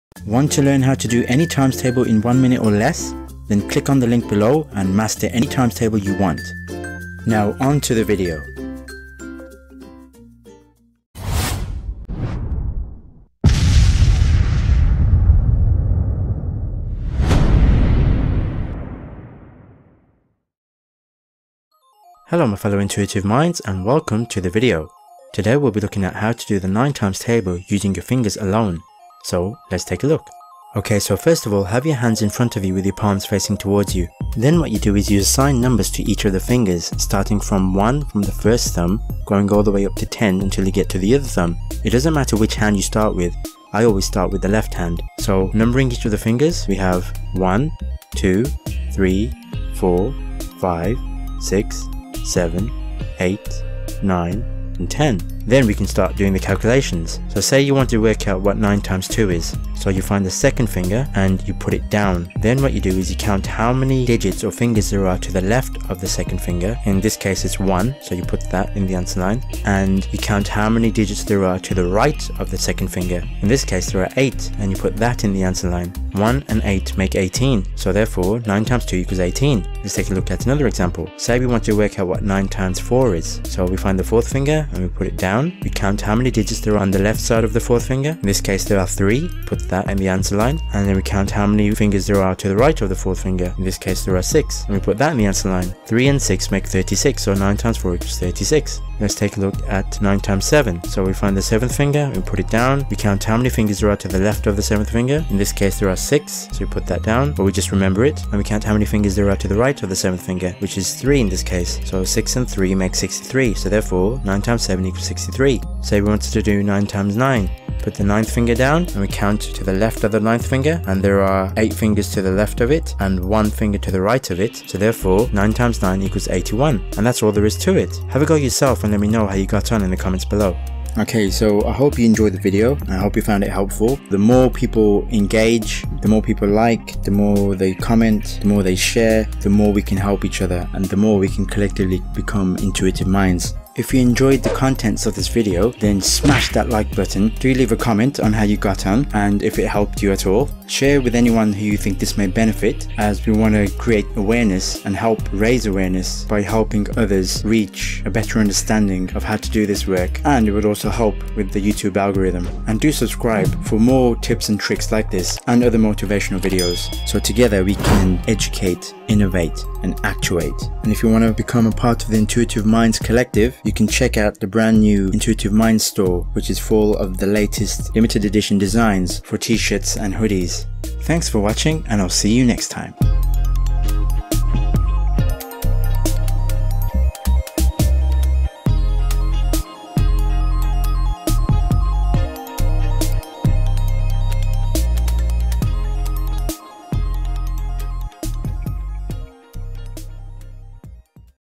Learn the 9 times table EASILY and FAST using your FINGERS!!!
Become apart of the Intuitive Mindz collective.

Having trouble remembering the 9 times table? Use this quick and effective trick to quickly learn, know and remember this times tables lightning fast.

Grab your interactive animated audio ebook here and learn any times table you want.